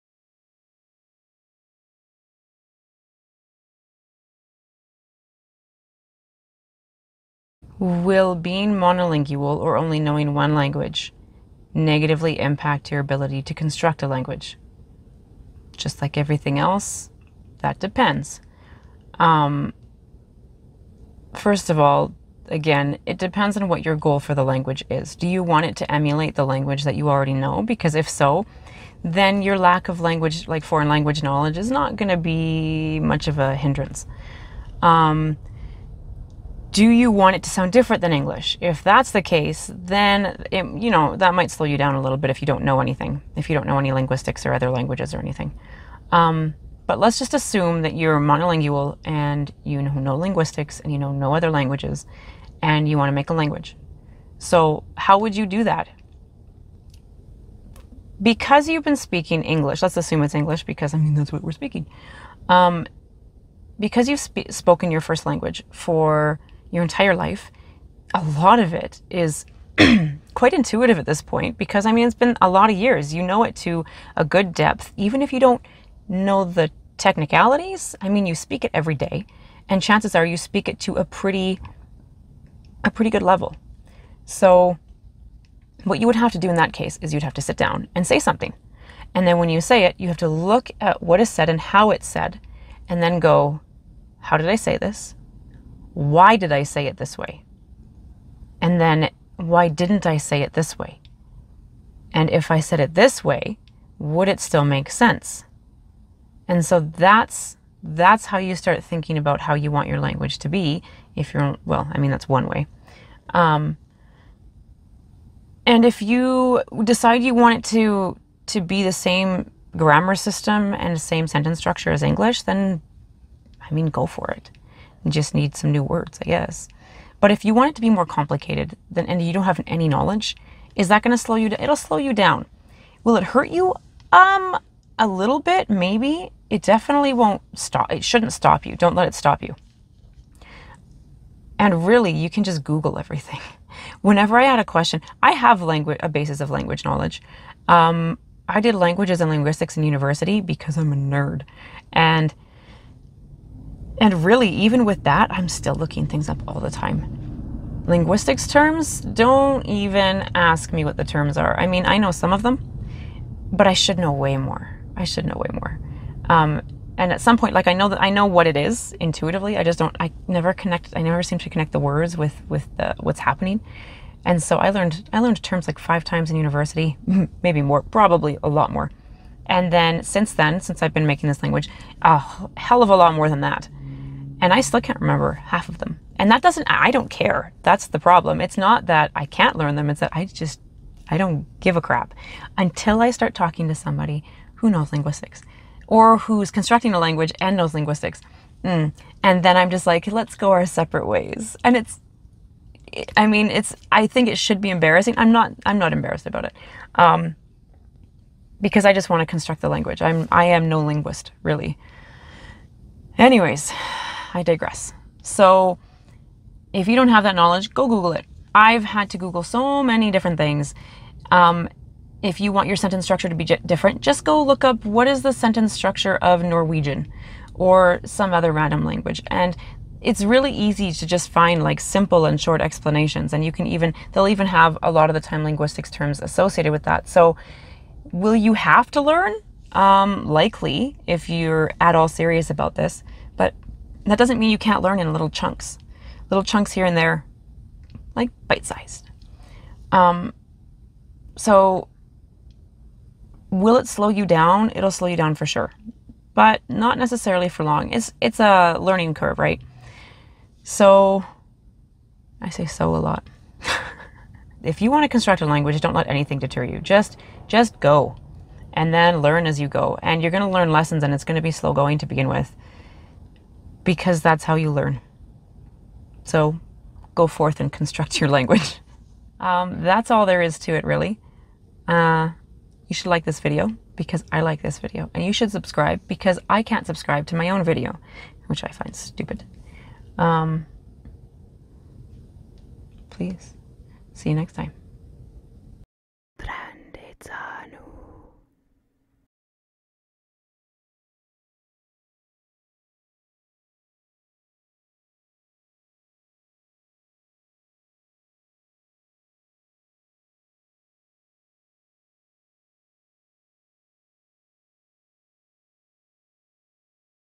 Can You Make a Language if You Only Know English?
Constructing a usable language is time-consuming and can be hard.
Can you still make a language if you only know one language and have never learned any linguistics?